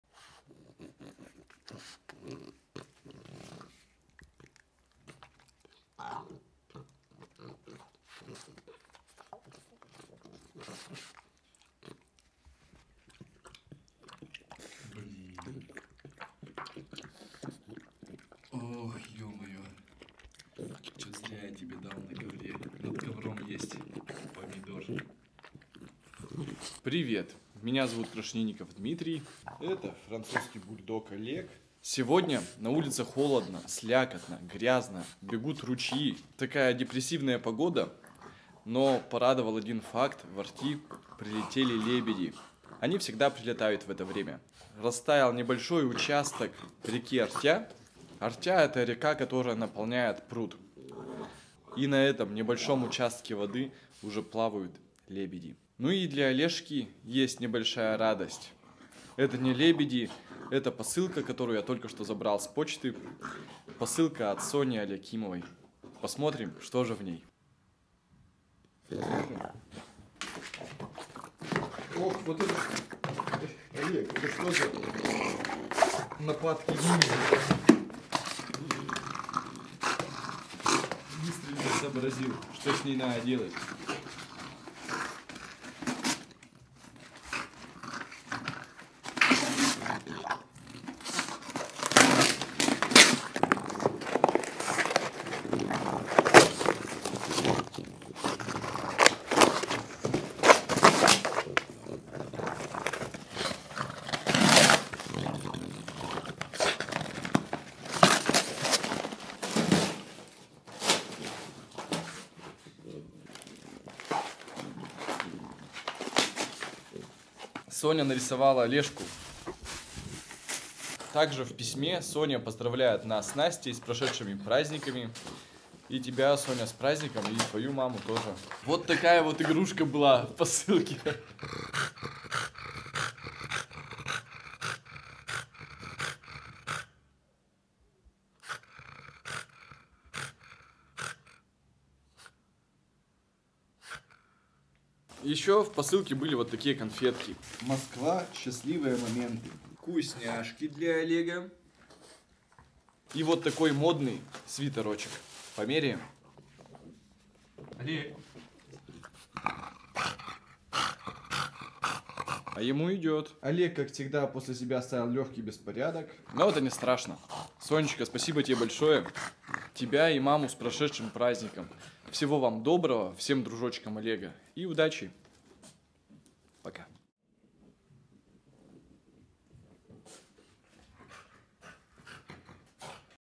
Зря дал собаке помидор... Французский бульдог Олег
Французский бульдог Олег сначала съел помидор, который я ему зря дал, а потом занялся распаковкой посылки от Сони Алякимовой.